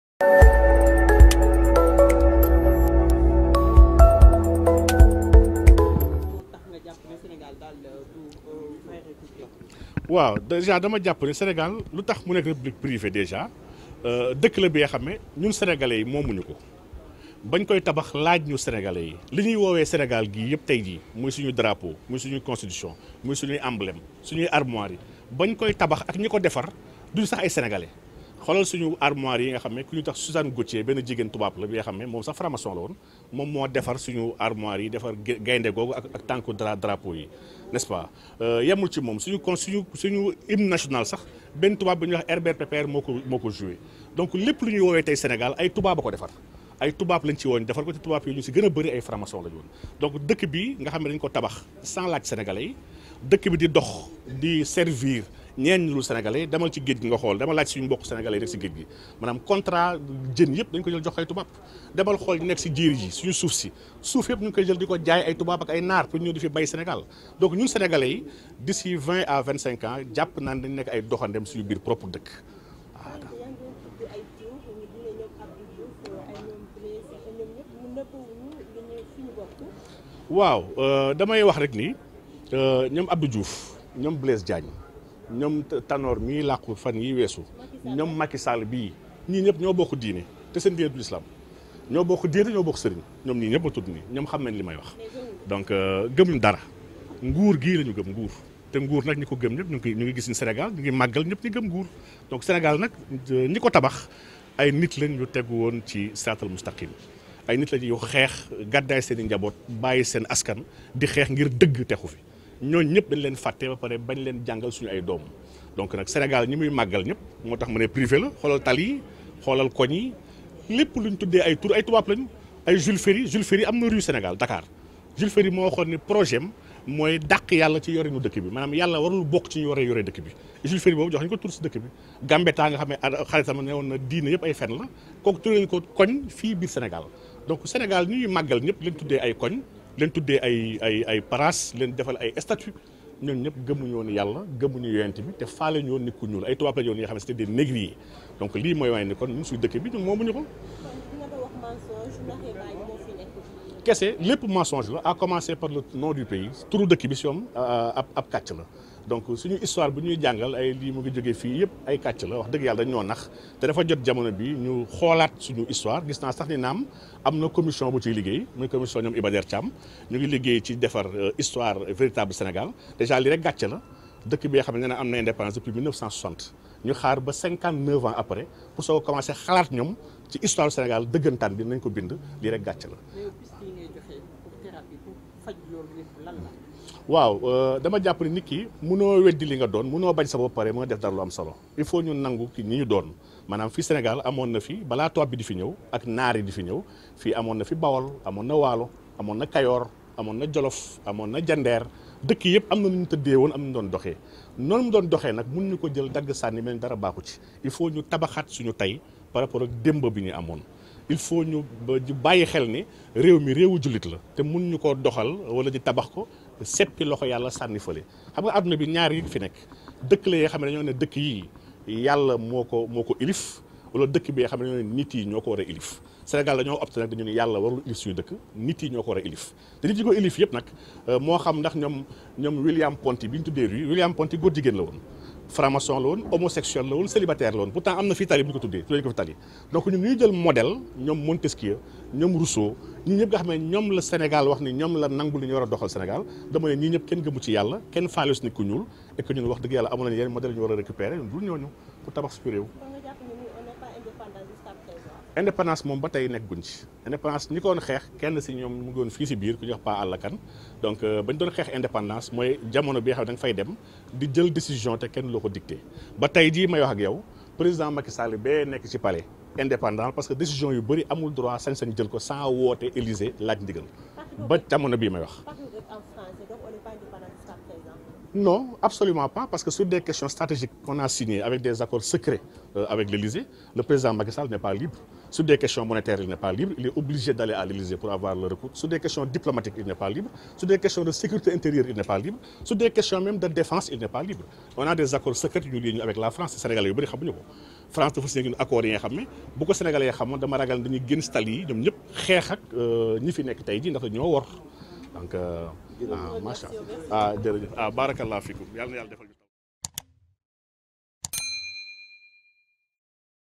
Cheikh Omar Diagne dénonce la présence des framaçons dans son livre
Enquête221TV, Le chemin de l 'information.